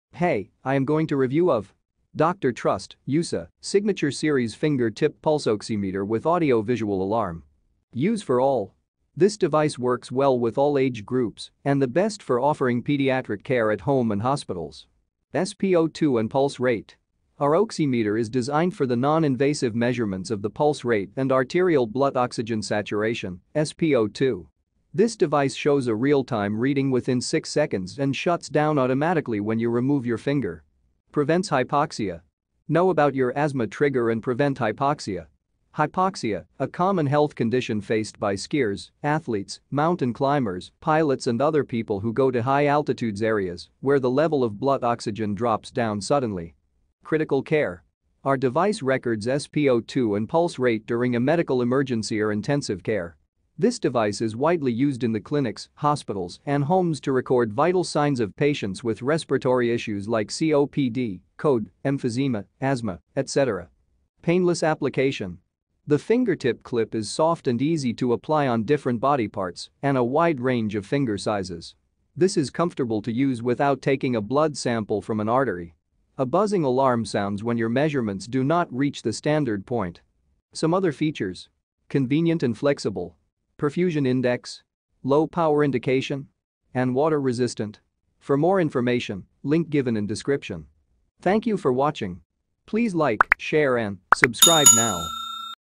Dr. Trust Signature Series | Review to buy online, Finger Tip Pulse Oximeter @ Best Price in India
Dr. Trust (USA) Signature Series | Review to buy online, Finger Tip Pulse Oximeter With Audio Visual Alarm @ Best Price in India

✳️Critical Care
✳️Convenient And Flexible
✳️Perfusion Index
✳️Use for All
✳️SPO2 & Pulse Rate